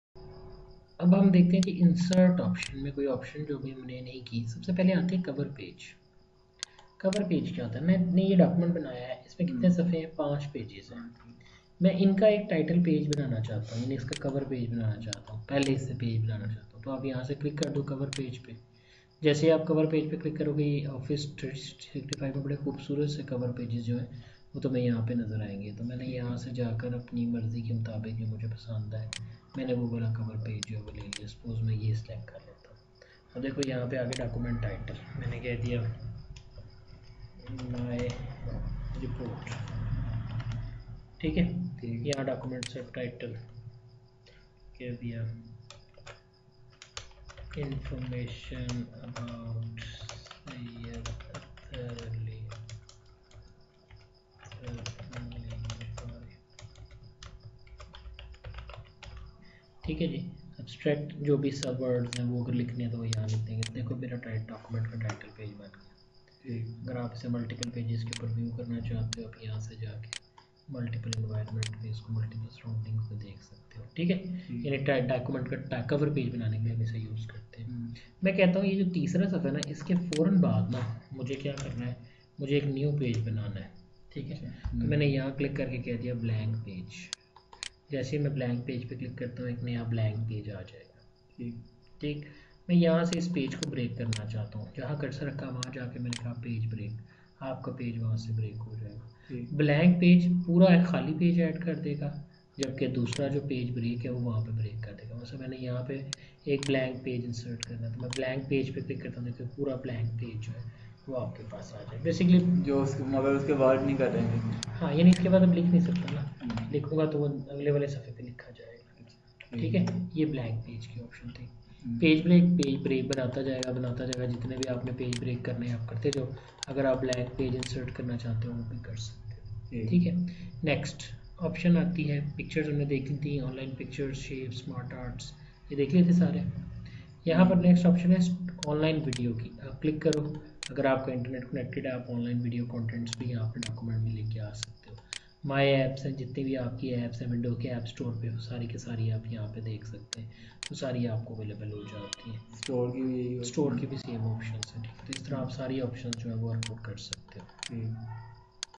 14. InsertPage Lecture 14:Trainings in Urdu
How to start working with windows 7.
Get control on your Personal Computer.
Control the basic Operating system with mouse and keyboard.
Make your self perfect with windows 7.
Press Like button, Share it on social media & give kind comments for betterment. Thanks in Advance. Regards...
By: Syed Ather Ali Bukhari